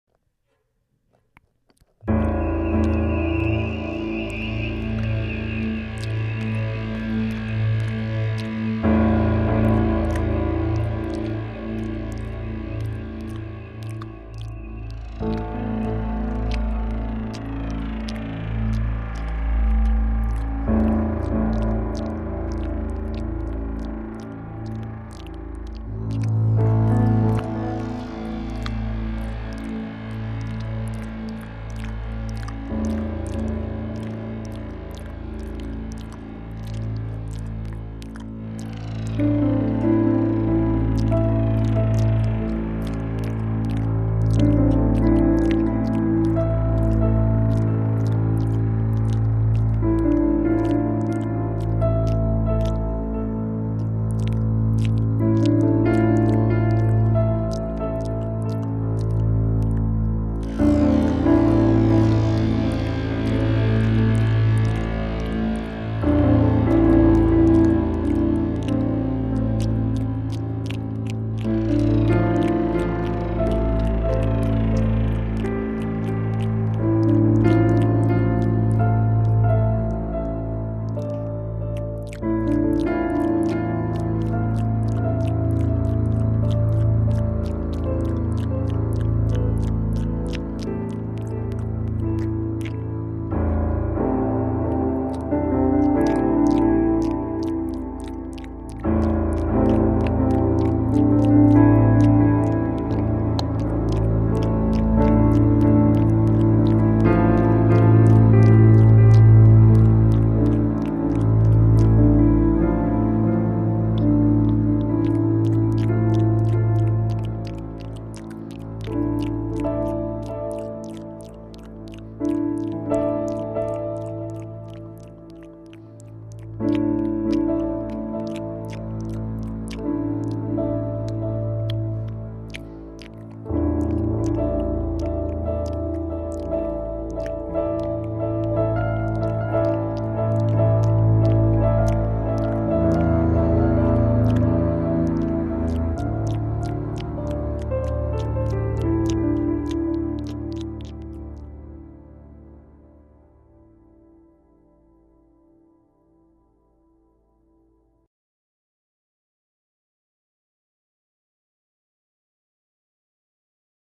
Walking Leaf Bug Phyllium is eating
leaf insect Phyllium sp 'philippines' eating leaves macro closeup close up.